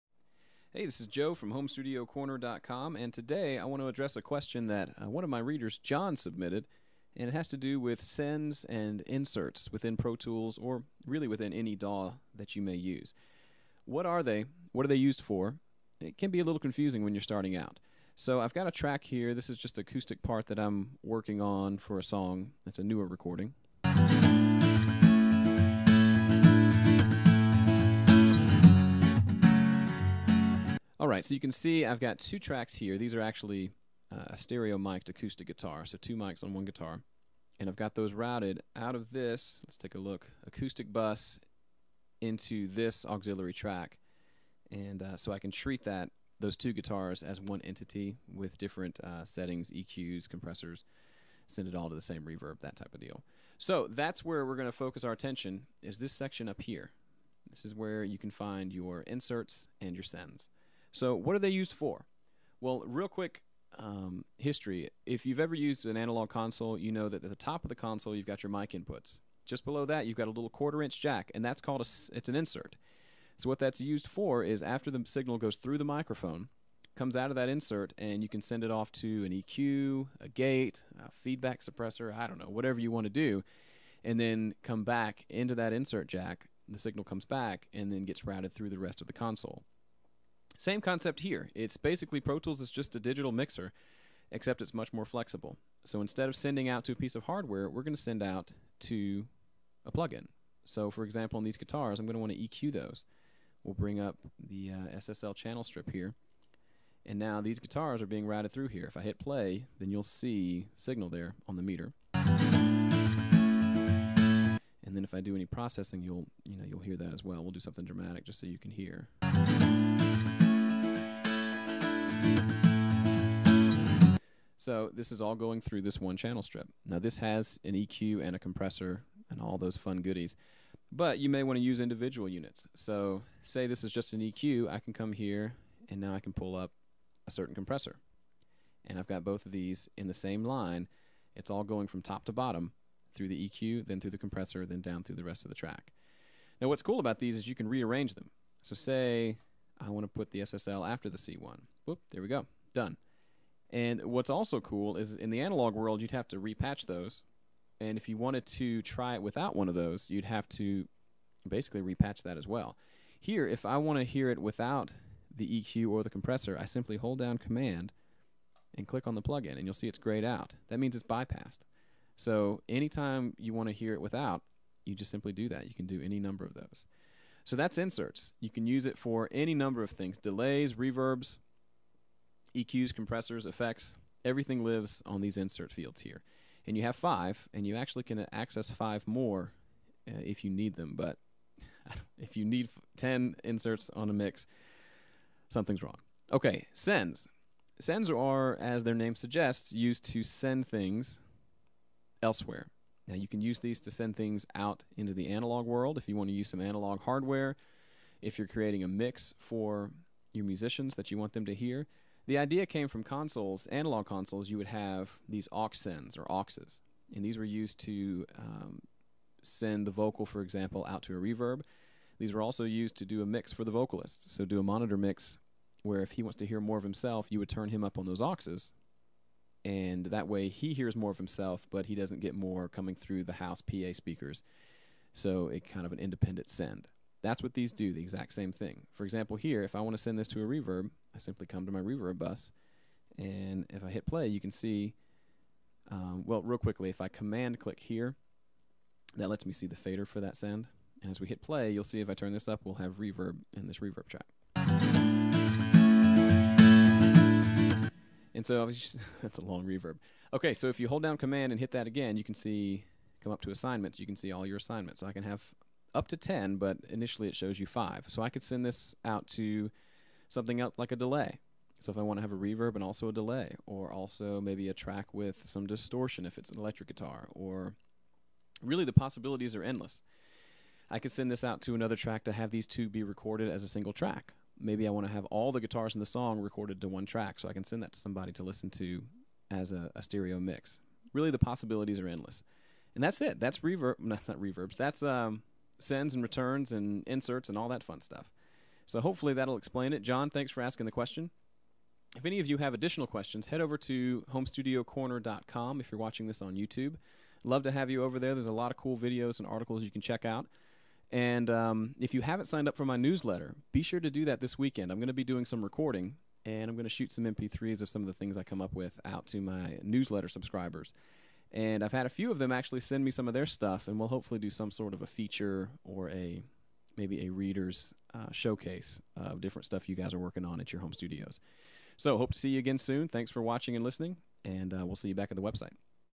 Sends & Inserts
💨 Join the Speed Mixing Challenge 💨
In this video I explain inserts and sends in Pro Tools.